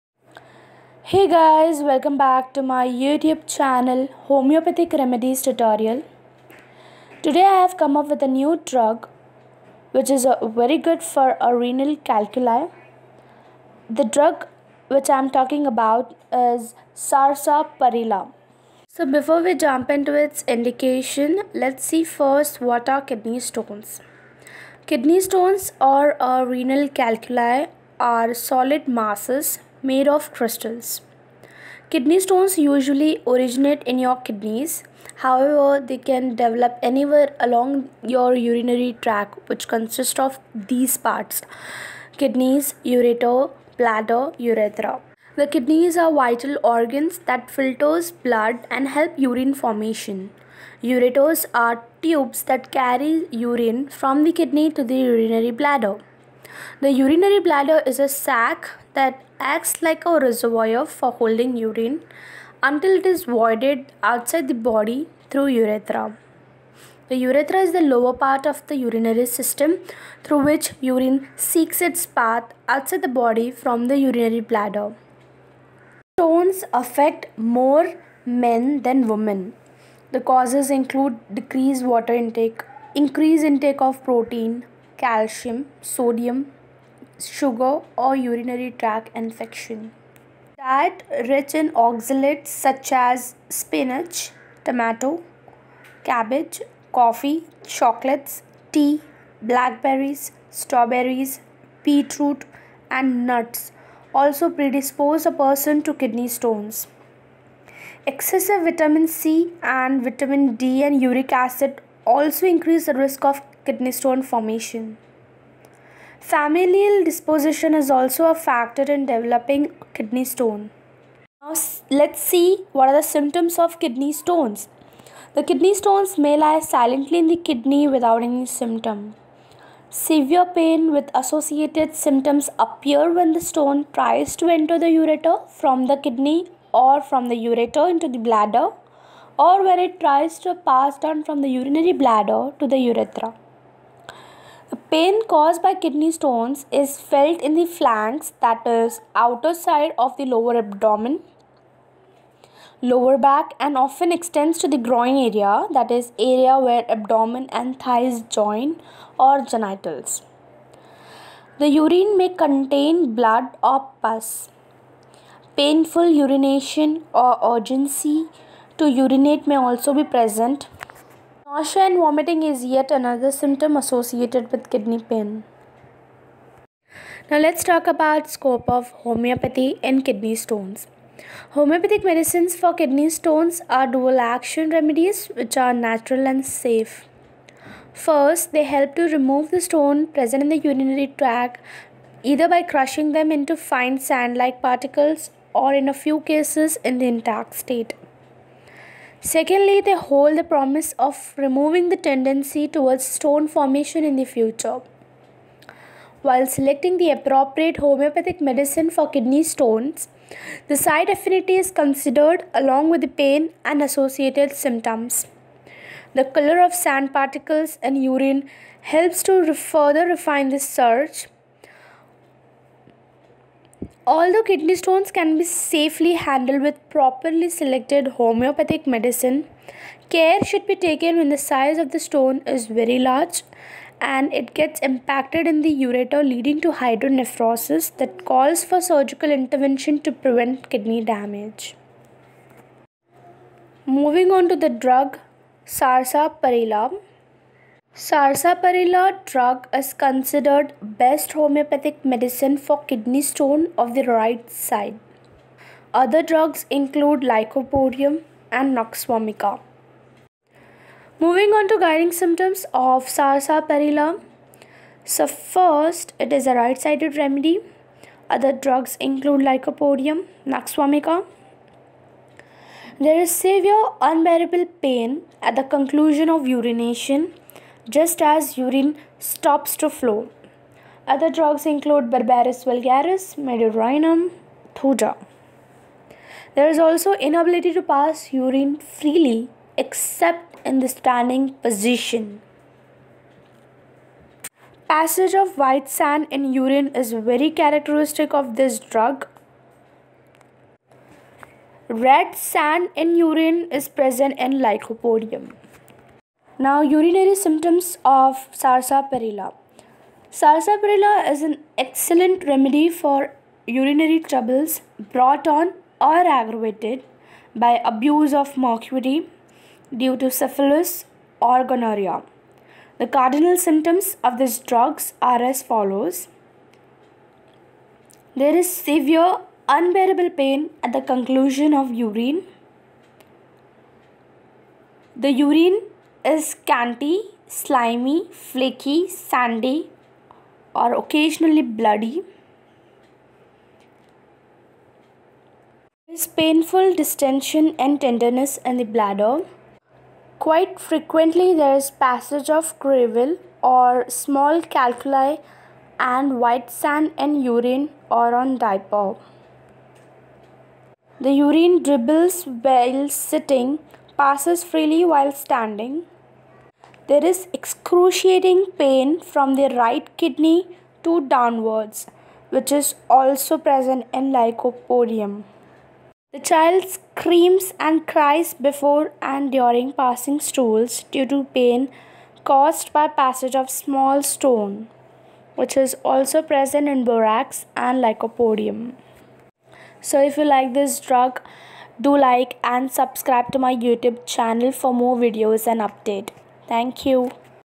SARSAPARILLA | BEST REMEDY FOR RENAL STONE || REMEDY FOR RENAL COLIC
SARSAPARILLA | BEST REMEDY FOR RENAL STONE || REMEDY FOR RENAL COLIC||Hello friends,
 Watch this video to know  Kidney stone.
I have explained the sign, symptoms and causes of kidney stones and also scope of homeopathy in kidney stone.
 Have given  brief explaination on sarsaparilla .